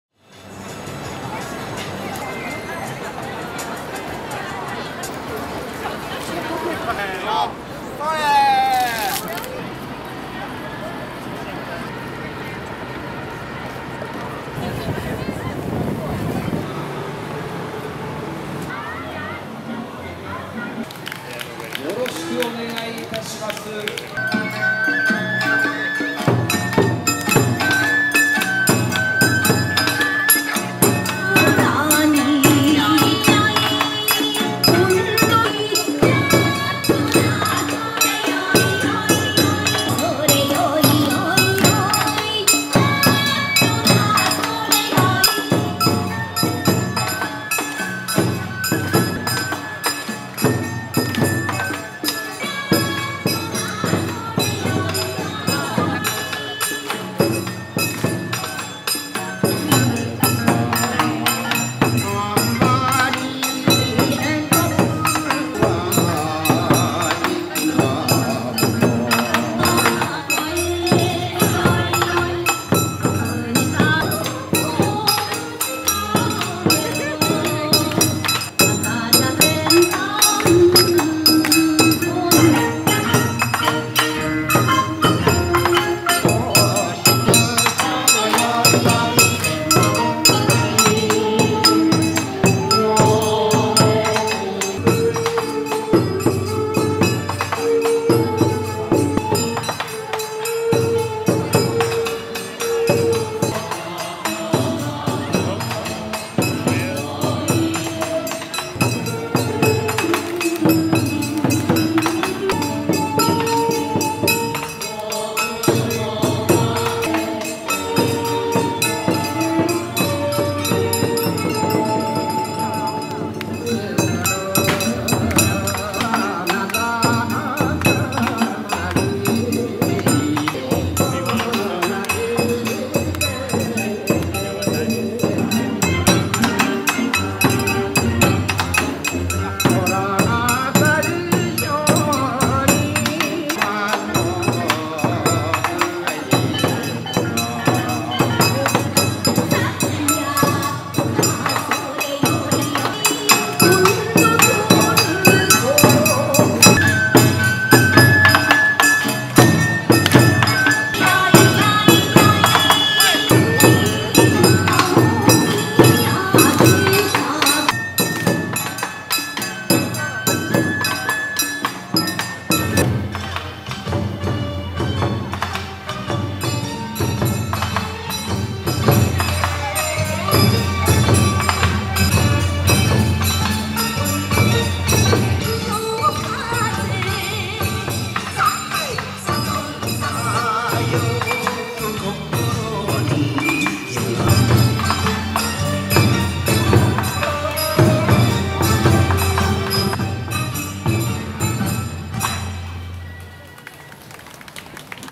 Excerpts from Marunouchi Bon Odori 2013「東京丸の内の盆踊り２０１３」
Excerpts from Marunouchi Bon Odori in 2013. This was my first attempt. It wasn't very successful with several cuts in the music. The 2014 version features a complete dance for about four minutes